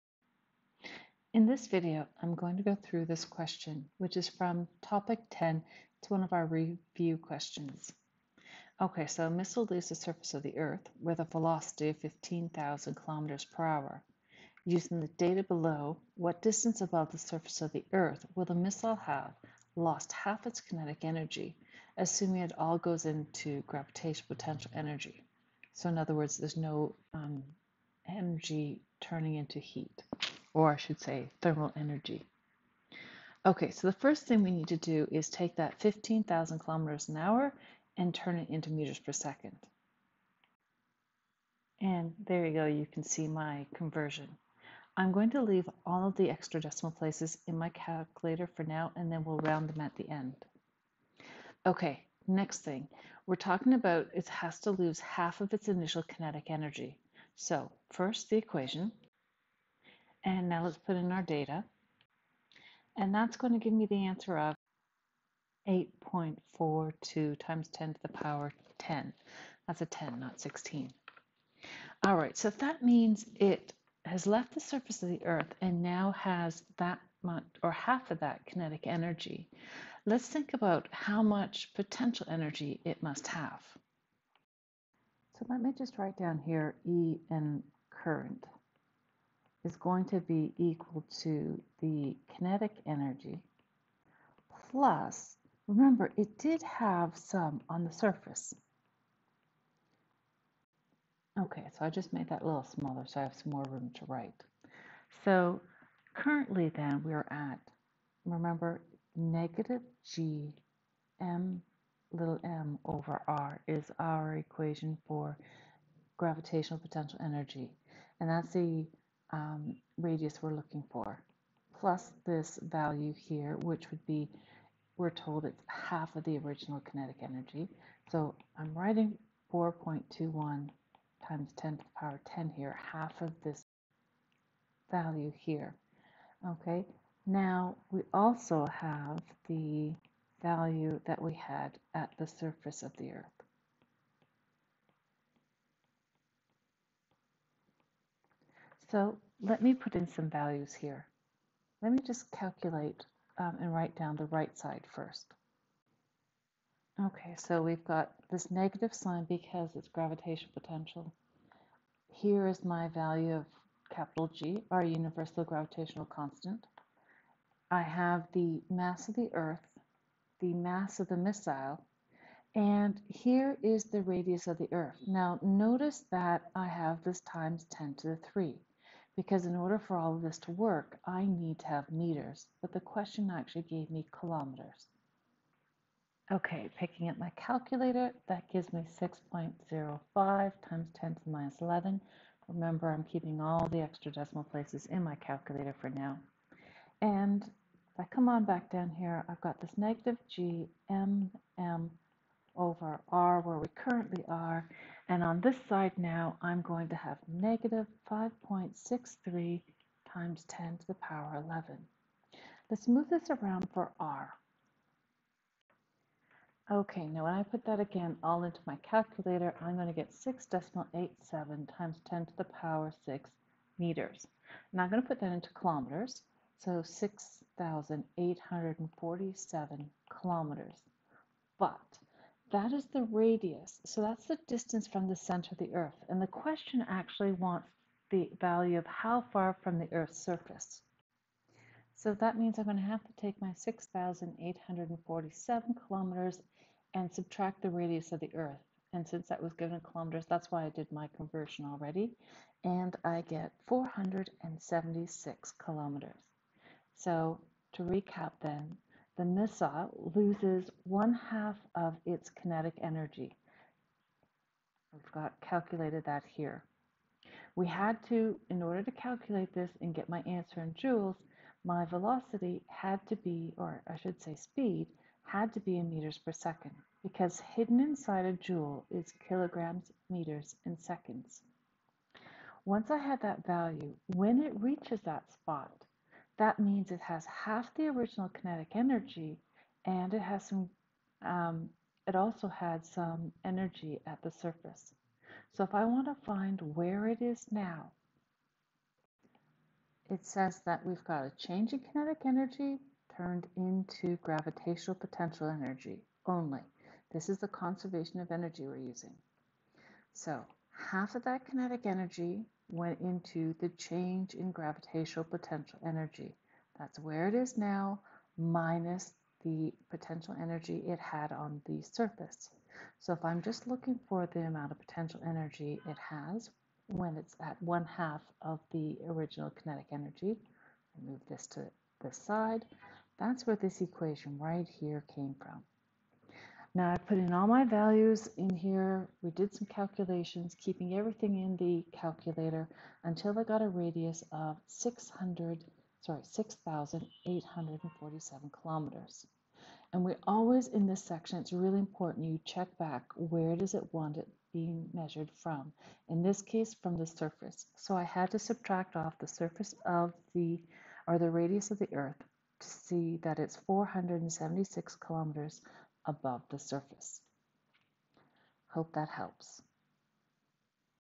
Topic 10 Review Question
This question includes the calculation of the conservation of energy of a missile that is leaving the surface of the Earth.  You must use gravitational potential energy as a negative value from Topic 10 of the IB Physics Higher Level course as "g" is only valid on the surface of the Earth.  This is not part of the SL course.